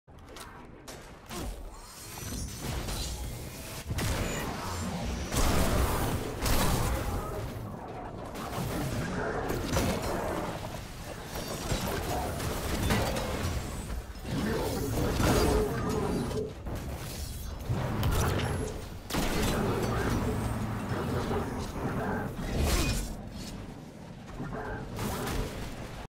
999,999 Damage with ONE hammer!
Buffs are: Unstable light, Roaring flames x3, scientific method, Burning fists x5